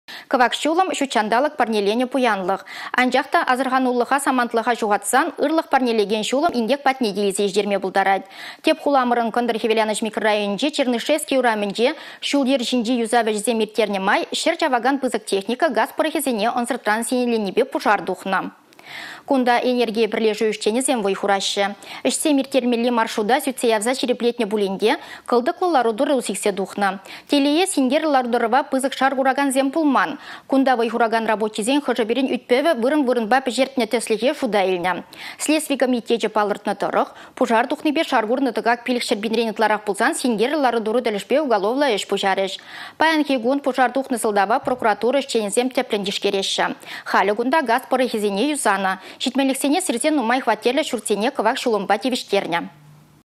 Кăвак çулăмран инкек
Кăвак çулăм – çутçанталăк парнеленĕ пуянлăх. Анчах та асăрхануллăха саманатлăха çухатсан  ырлăх парнелекен çулăм инкек патне те илсе çитерме пултарать. Тĕп хуламăрăн кăнтăр-хĕвеланăç микрорайонĕнчи Чернышевски урамĕнче çул-йĕр çинче юсав ĕçĕсем ирттернĕ май çĕр чавакан пысăк техника газ пăрăхĕсене ăнсăртран сиенленипе пушар тухнă.